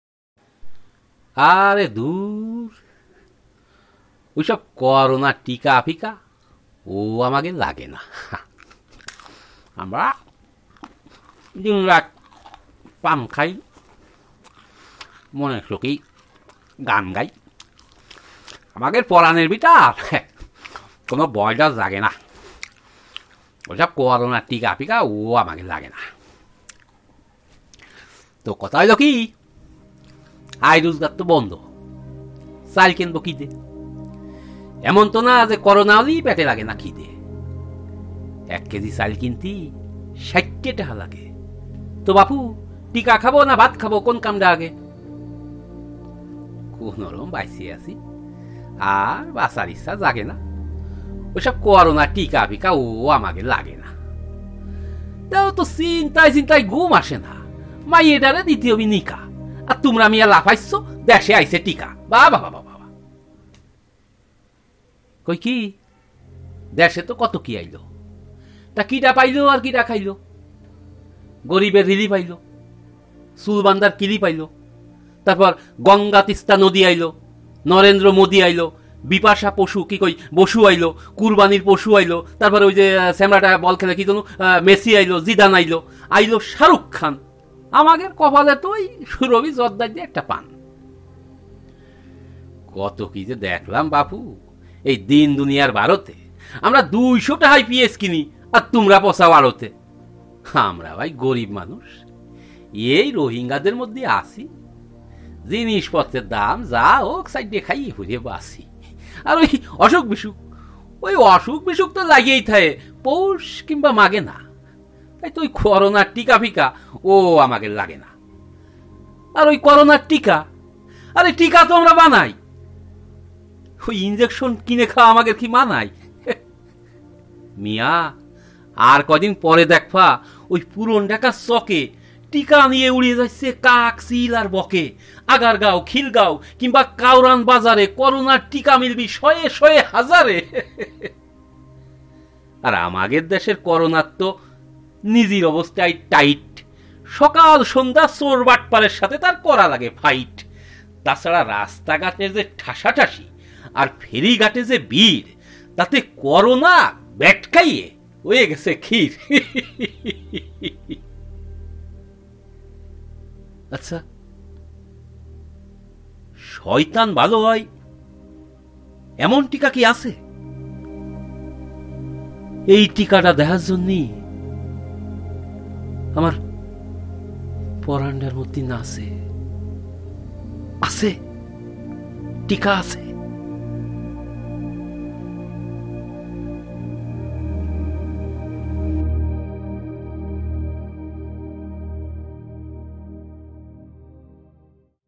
একটি করোনা মুক্ত ভাইরাল কবিতা
Title : করনা মুক্ত কবিতা, ভাইরাল কবিতা, বাংলা কবিতা,

কবি : অহেতুক মুখোপাধ্যায়
আবৃতি : বিব্রত বঙ্গপাধ্যায়

Ownership and credit:
DTN Bangla Ltd.

::::::::::::::::::::::::::::::::::::::::::::THE END ::::::::::::::::::::::::::::::::::::::::::::::